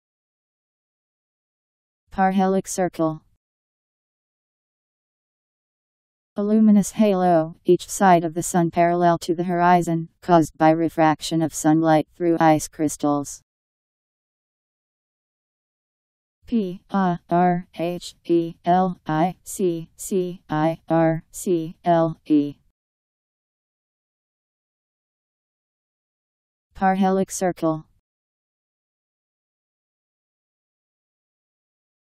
Parhelic circle Meaning
Video shows what parhelic circle means. A luminous halo, each side of the sun parallel to the horizon, caused by refraction of sunlight through ice crystals..  Parhelic circle Meaning. How to pronounce, definition audio dictionary. How to say parhelic circle. Powered by MaryTTS, Wiktionary